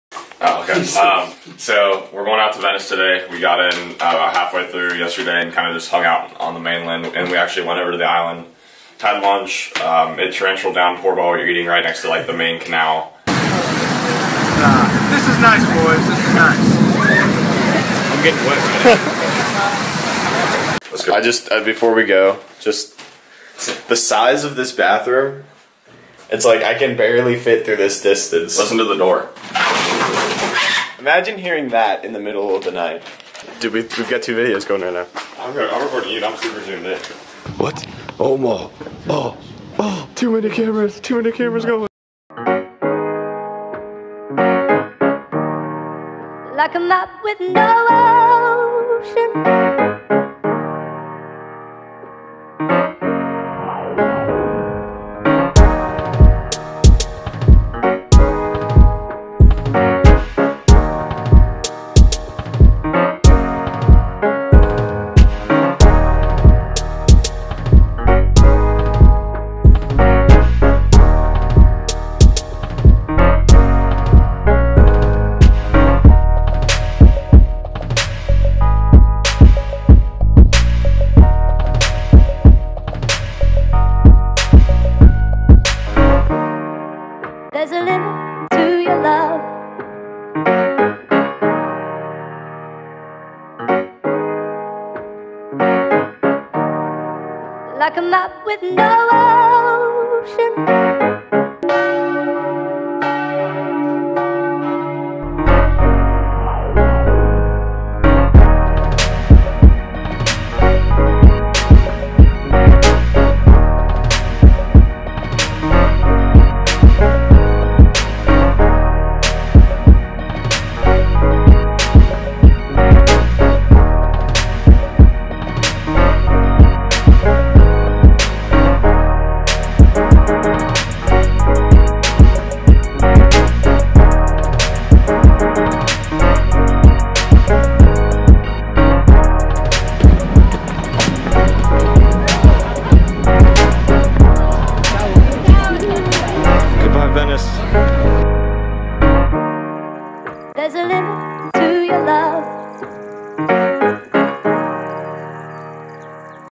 A Summer Day in Venice | Senior Trip - Europe 2017
On our trip we stopped in Venice for a day and a half. The first night we got there we took a train onto the island and stopped for dinner next to the grand canal. Soon afterwards, it started pouring down rain for a good half hour. We waited until it stopped and then headed back to the hostel. The next day, we took the journey back to the island and spent the day exploring. It was the perfect summer day, and where better to spend it than Venice.

Filmed on a Lumix g85 w/ 12-60mm and some shots on a Sony a6000. Edited on Final Cut Pro X with mLut color grading.